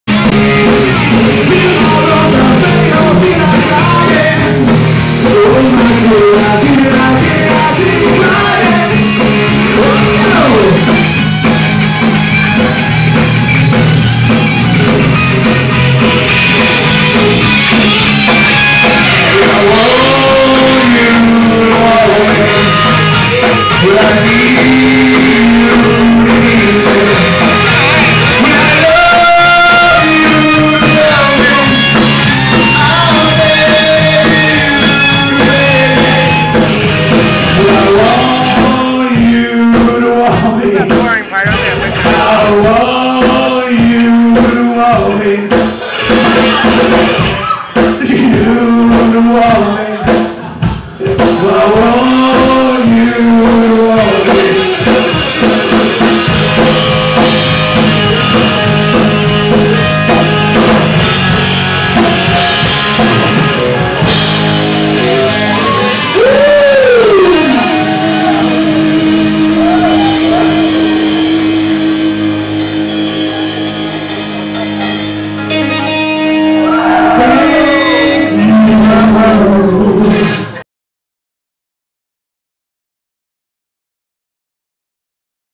Dive Bar Fort Lauderdale Florida Feb 5, 2011 12:26 AM
.Dive bar in Fort Lauderdale, Florida. Always a good time. No matter what. Band playing is called Self Induced.